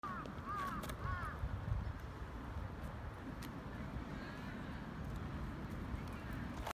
Deer Bows For Food In Japanese Park
ID: 1504329

A tourist was left thinking 'bow’s about that?' after a sacred deer repeatedly bowed to him in a Japanese park.

Traveller Daniel Speight, 26, was visiting Nara Deer Park, south of Kyoto, last month when he captured the bizarre animal encounter.

The IT technician, from Loughborough, UK, couldn’t believe his eyes when the tame mammal dipped its head to him over and over again in return for food.

Daniel said the deer have learned to bow to humans after years of Japanese people, who believe them to be sacred, doing the same to them.